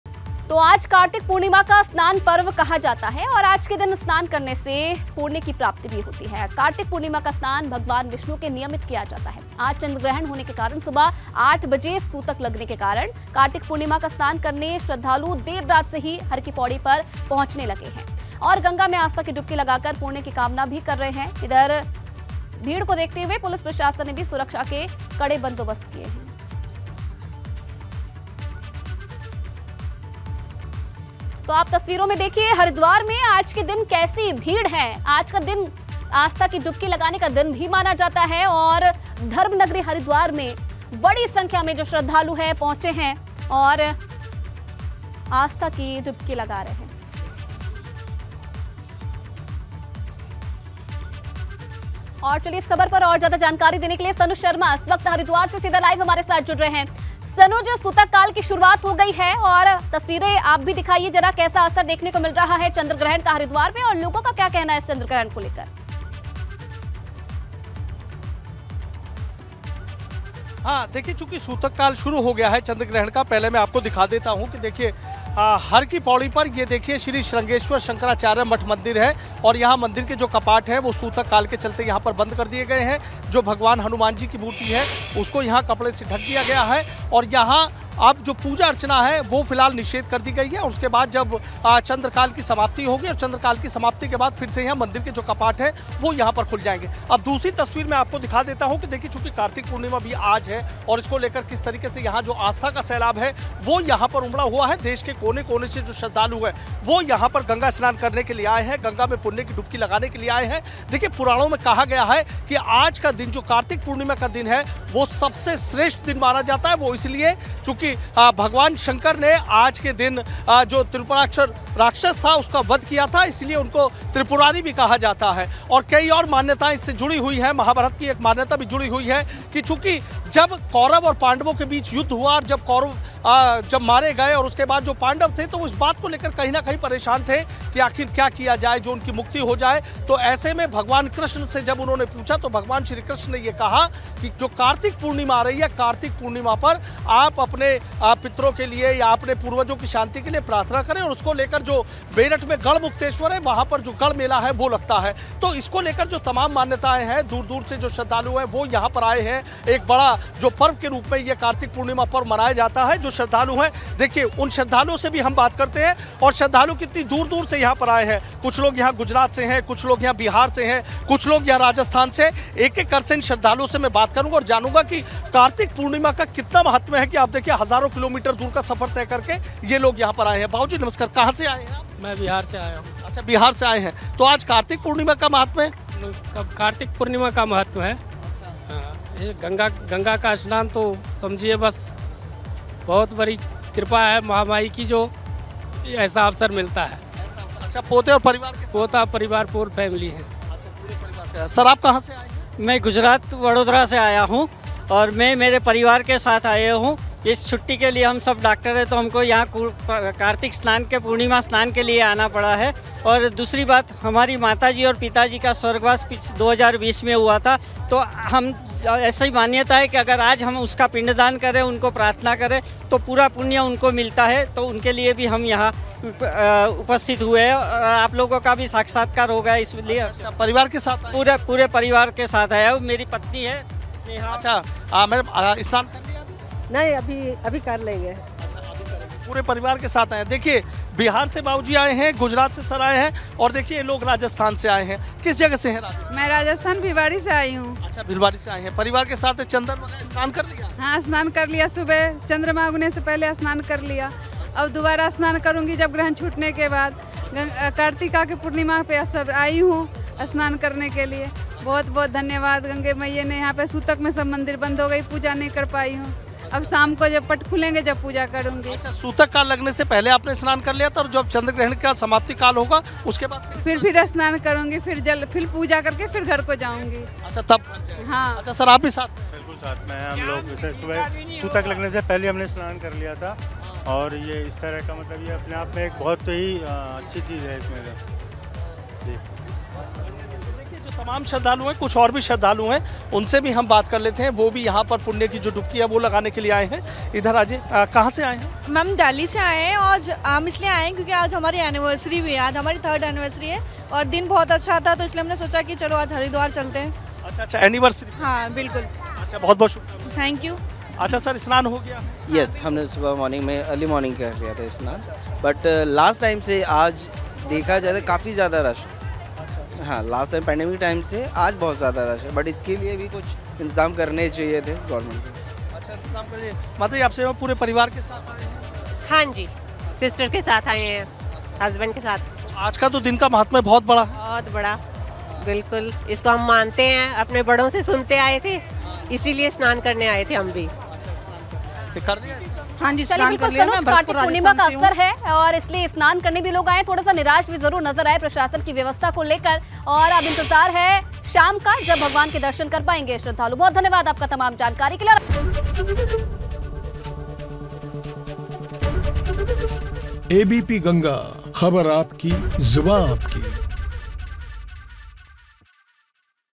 Uttarakhand: कार्तिक पूर्णिमा का स्नान करने हर की पैड़ी पर उमड़ी श्रद्धालुओं की भीड़
Uttarakhand: कार्तिक पूर्णिमा का स्नान करने हर की पैड़ी पर उमड़ी श्रद्धालुओं की भीड़, बता दें आज चंद्रग्रहण के साथ कार्तिक पूर्णिमा भी जिसे लेकर उत्तराखंड में हर की पैड़ी पर सुबह से श्रद्धालुओं की भारी भीड़ देखा जा रहा है..देखिए